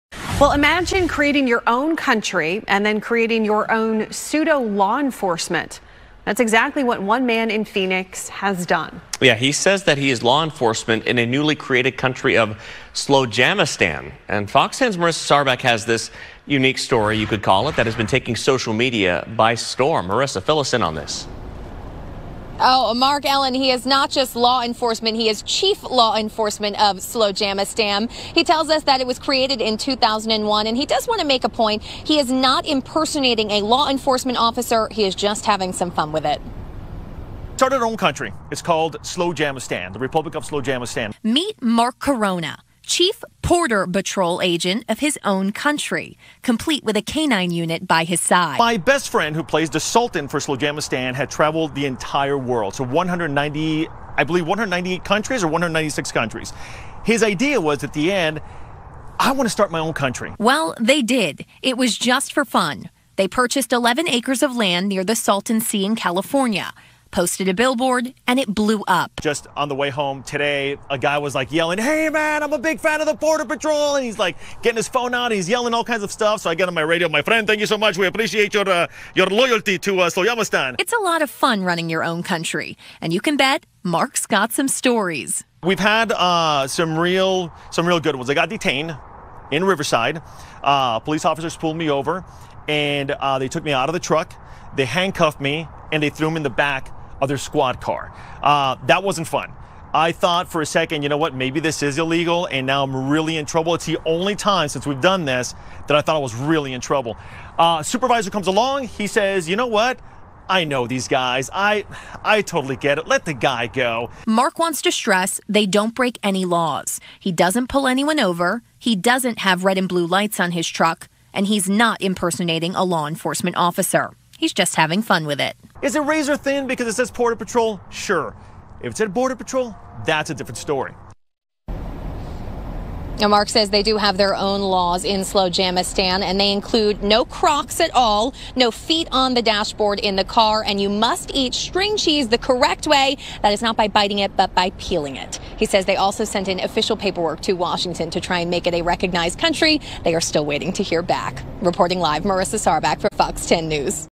Porder Batrol on Fox News - Slowjamastan
"It's a car that has caught the attention of a lot of people, and the Arizona man who owns a truck has a rather unique story behind the vehicle.

As it turns out, Mark Corona is not trying to impersonate Border Patrol. The vehicle does not have those flashing lights that law enforcement vehicles have, and Corona also does not try to pull people over.

The car, according to Corona, is actually part of a 'country' called 'Slowjamistan.'

Since The "Porder Batrol" truck has made the news, copycats have surfaced, including a Florida man who drives a "Booty Patrol" truck.

FOX 10's Marissa Sarbak has more on a story that is rather out of the ordinary."